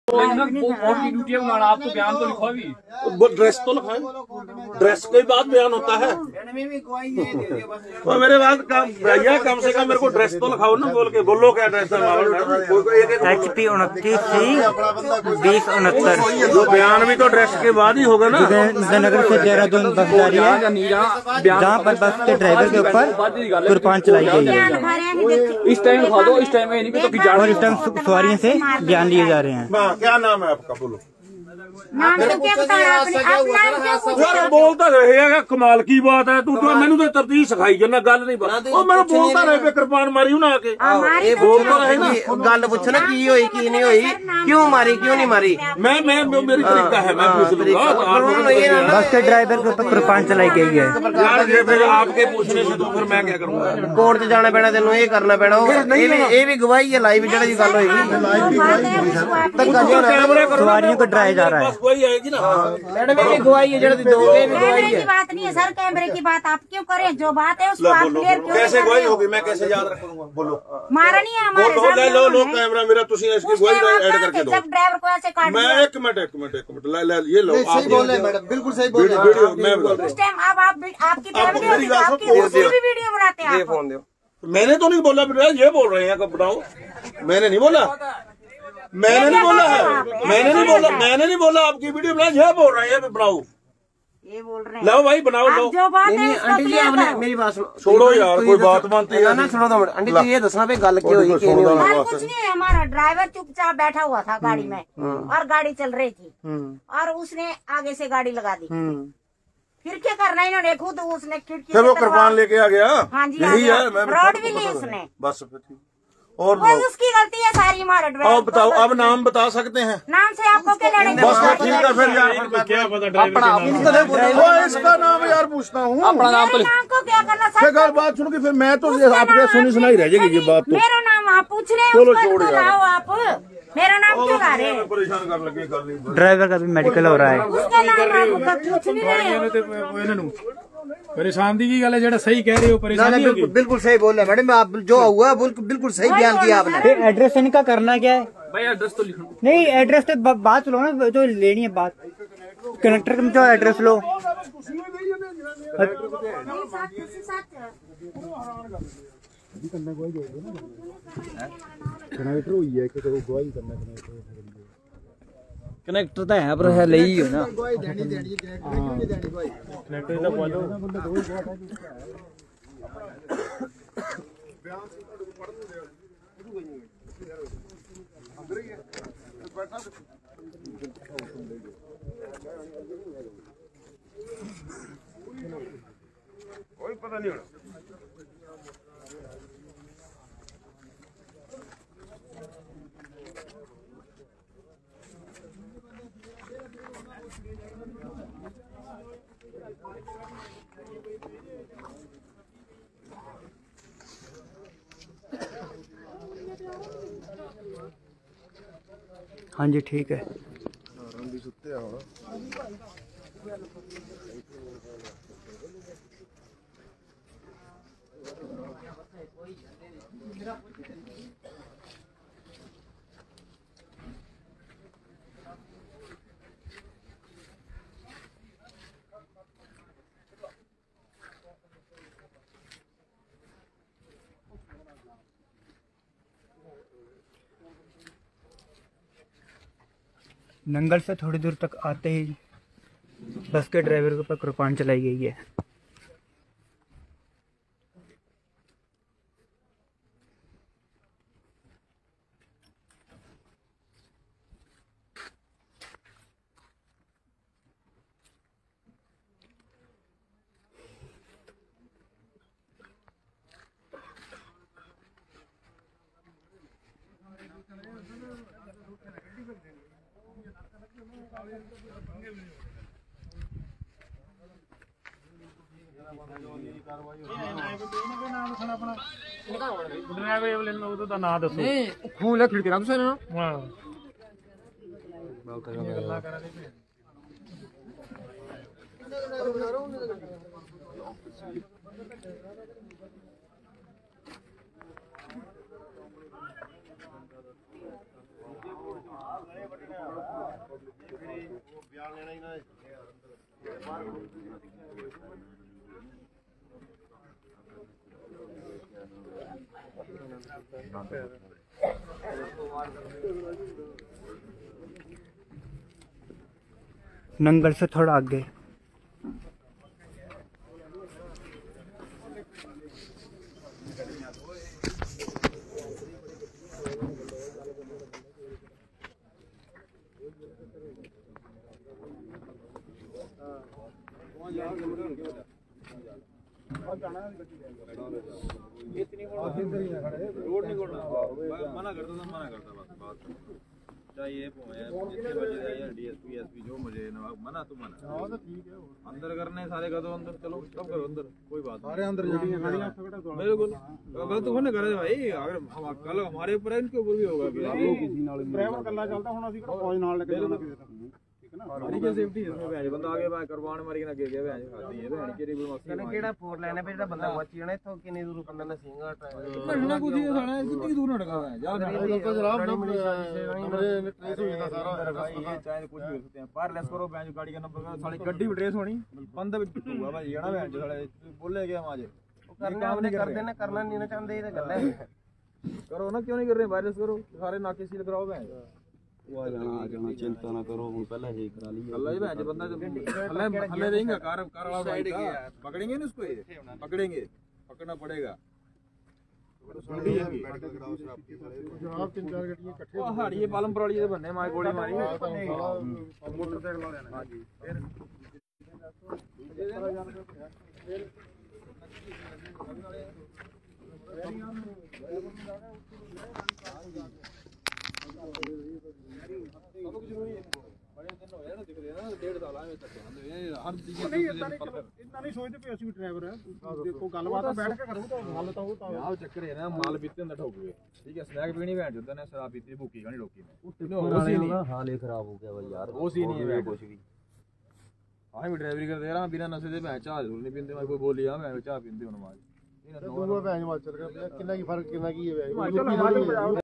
HRTC बस के चालक के साथ पंजाब के टिपर चालक ने कृपाण से हमला किया
बस नंबर HP29 C 2069  जो कि जोगिंदरनगर से देहरादून जा रही थी बह बस अभी नगल से थोड़ा आगे हे पहुची थी कि  HRTC बस के चालक पर PB65BC - 7960 t
टिपर चालक ने कुर्पान के साथ हमला कर दिया।